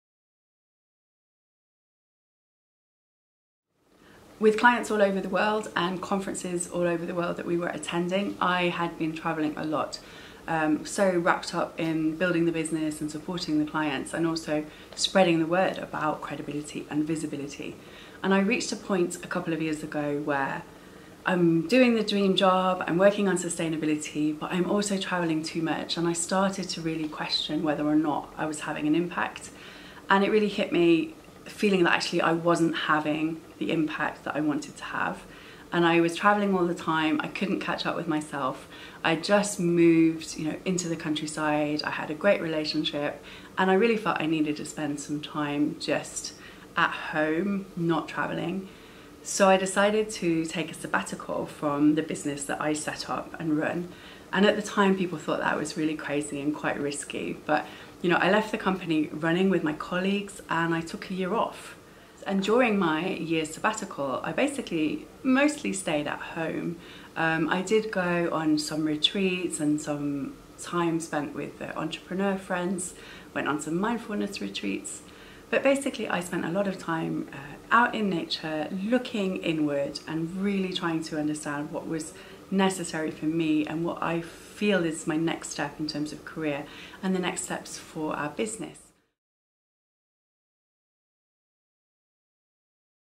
CEO interview: Why did you take a sabbatical?
Kathryn Sheridan, CEO of Sustainability Consult, discusses her year out and why she took it.